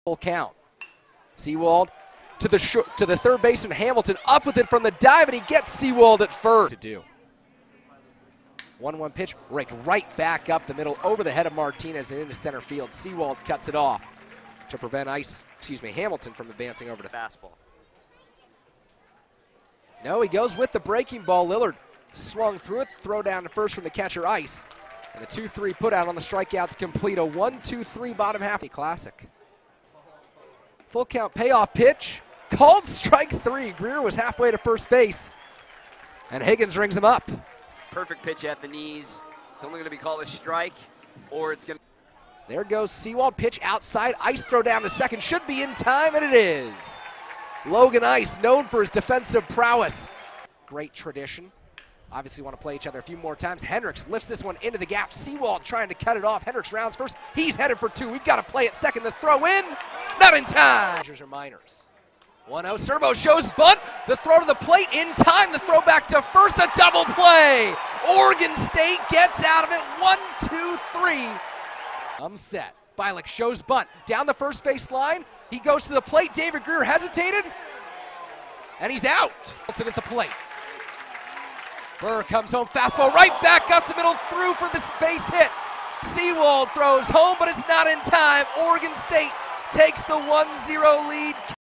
OSU Baseball Defeats Arizona State, 1-0
The Oregon State Baseball team defeated Arizona State, 1-0, at Phoenix Municipal Stadium on March 13, 2015.